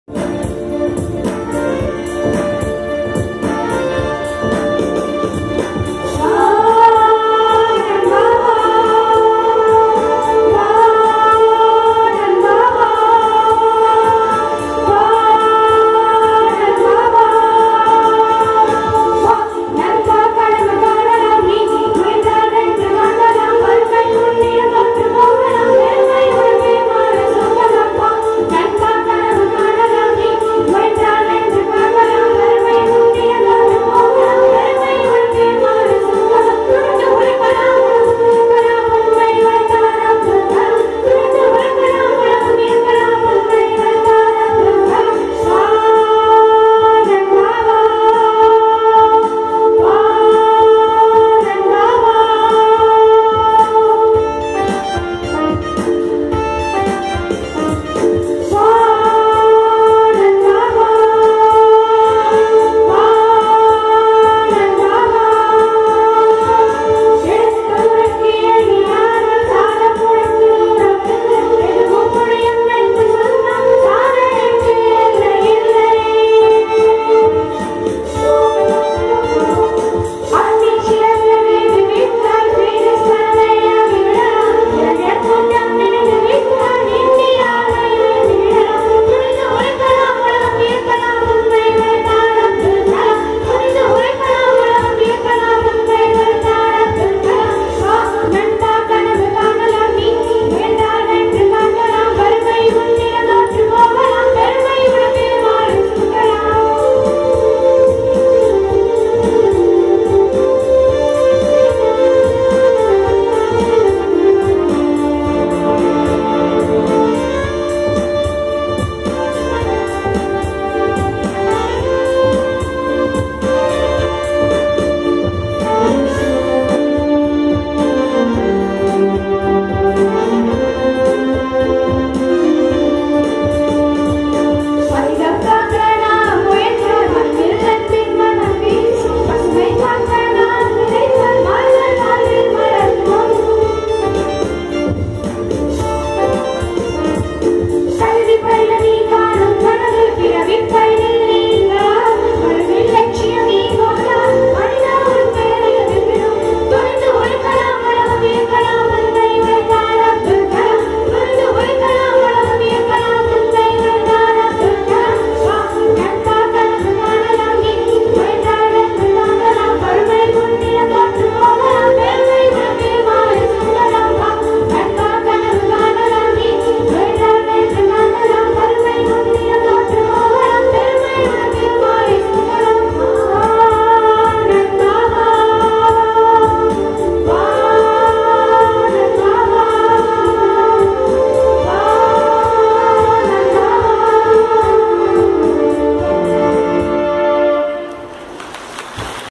Dr kalam's birthday songs
Students of Syed Ammal matriculation school ramanathapuram salute their acknowledgement to our man of saint Dr APJ KALAM by their talents by 11a.m.